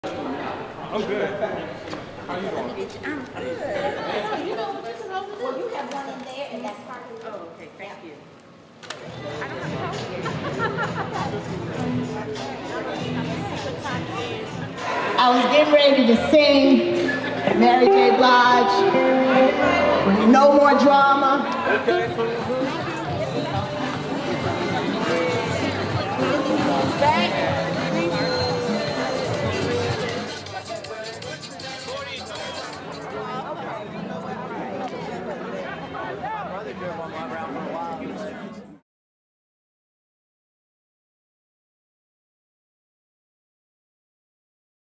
Clips from WTU's Teacher Appreciation Party!
Thanks to the many teachers who helped us to celebrate them at WTU's Teacher Appreciation Party on Friday, May 5th! @WTUTeacher RedforEd Teachers Unions @MayorBowser @DCPSChancellor